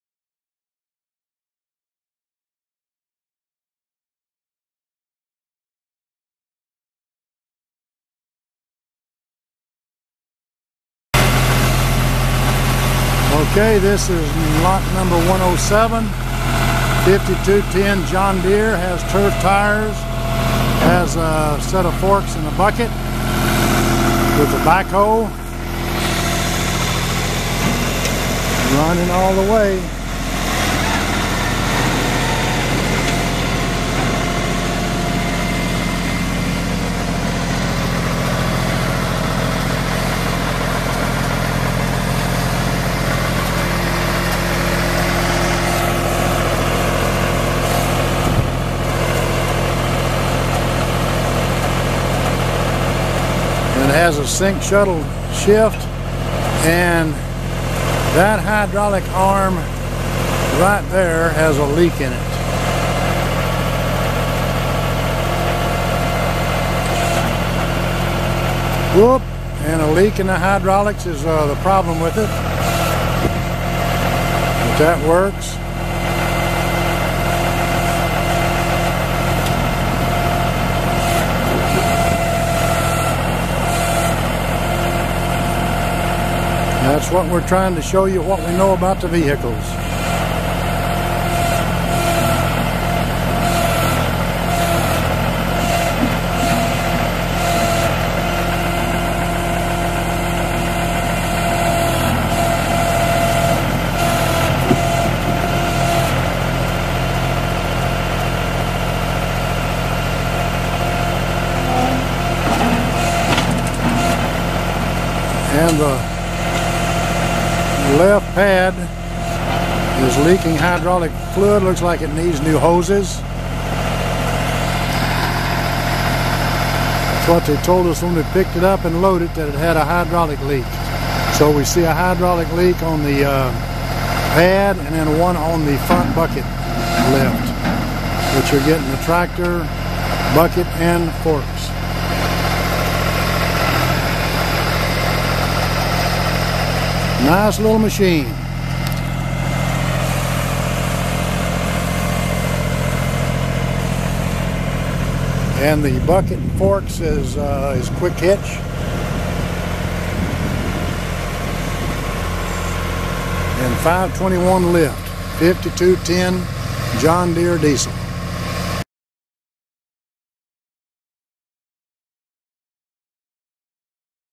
#107 2001 John Deere 5210 SWFWMD Vehicle and Equipment Public Online Auction
For your consideration a 2001 John Deere 5210 offered at public auction by Atkinson Realty and Auction. This tractor along with many other vehicles as well as equipment belonging to SWFWMD will be auctioned online, being sold to the highest bidder. and register today.